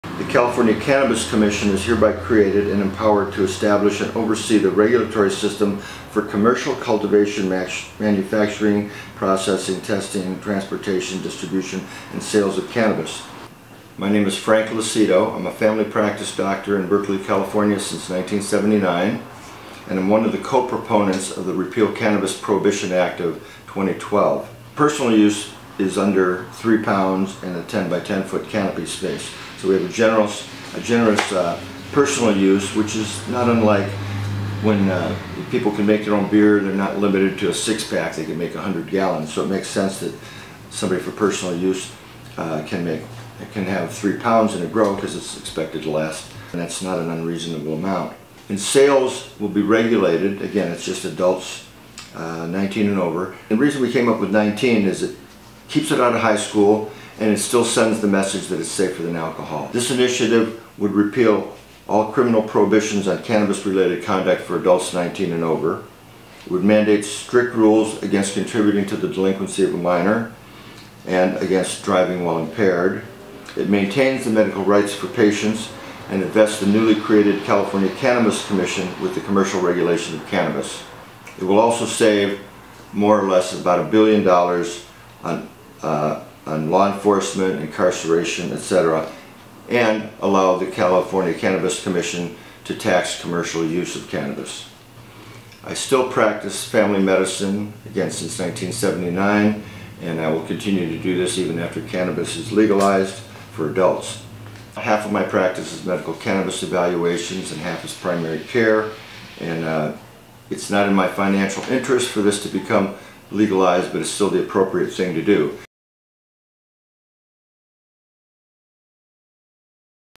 Dr. Frank Lucido Discusses Repeal Cannabis Prohibition Act
The  Regulate  Marijuana  Like Wine Act  of 2012
The  People of  the  State of California do  enact as follows:
Section  1.  Findings, Declarations, Purpose, Directives, and Orders
Section 11420 is added to the Health and Safety Code as Chapter 6.8 Regulation and
Taxation of Marijuana. This section shall be  known as and may be cited as the "The
Regulate Marijuana Like Wine Act of 2012," known hereinafter as the "Act."
(a)  The  People of the State of California find and declare all of the following:
(1) Outlawing marijuana has created illicit markets,  empowered gangs, drug
cartels and terrorists, resulted in violence,  corruption and violations of rights
against search and seizure, and contributed to the highest incarceration rate in
the world.